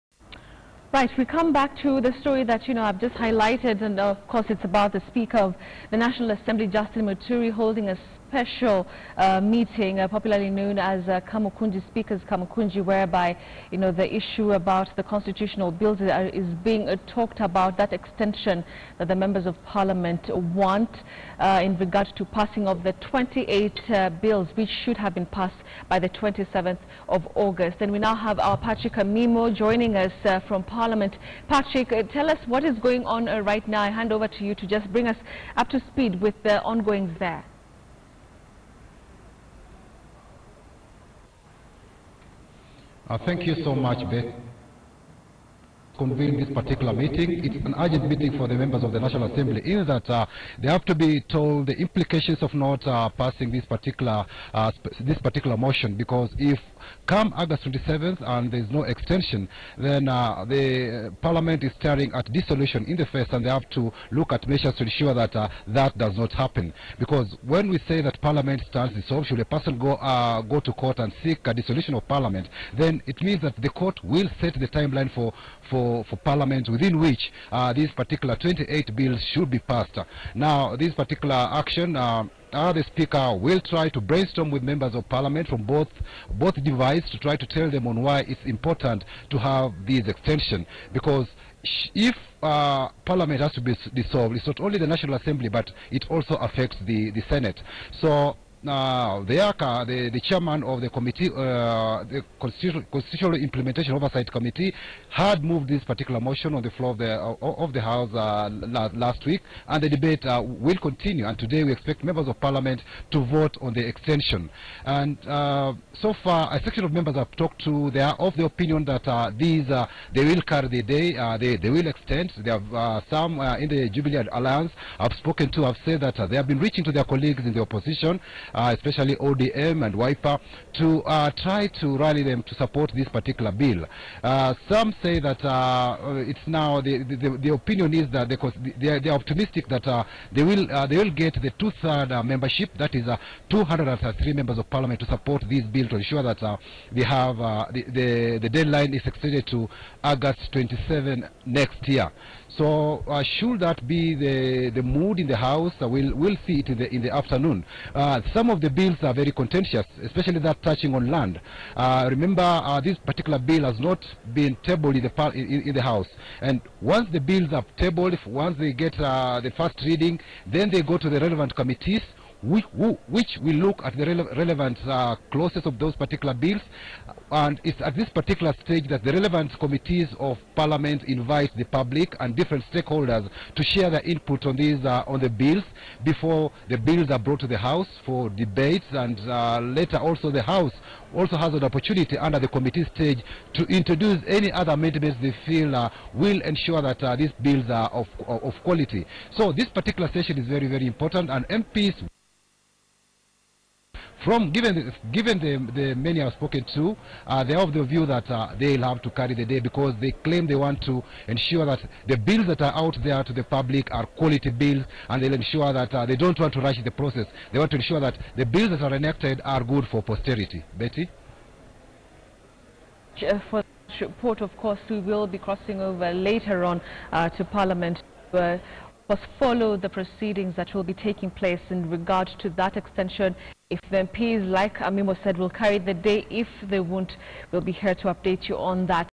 Why National Assembly has to extend the date for Constitutional Bills
The National Assembly will this afternoon continue debating a motion seeking to extend the August 27th constitutional deadline within which at least 28 bills have to be passed.  The special motion was moved by the chairman of the constitution implementation oversight committee Chairman Njoroge Baiya after it became apparent that parliament will not manage to pass the laws ahead of the five year deadline.  The government wants to extend the timeline by one year.  Parliament faces a risk of dissolution for failing to pass the laws within the stipulated timelines.